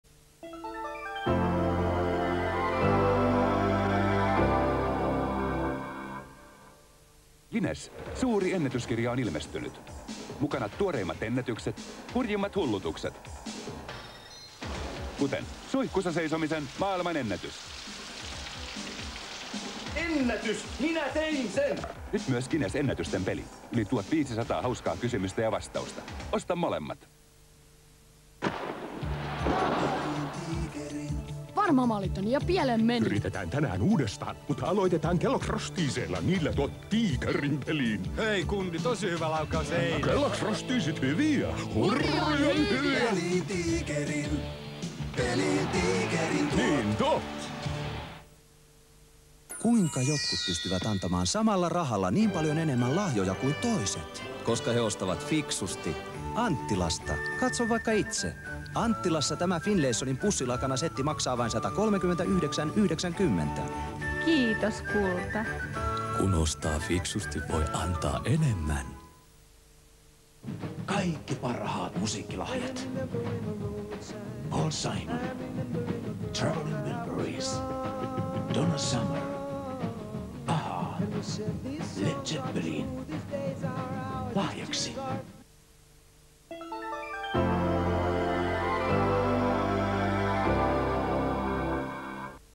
Mainoskatko - Finnish TV-commercials (1990) #3
Joulukuussa 1990 mainosteeveestä tulivat seuraavat mainokset:
00:06 Guinness Ennätyskirja 1991 ja lautapeli
00:28 Kellogg's Frosties
00:49 Anttila
01:10 Warner Music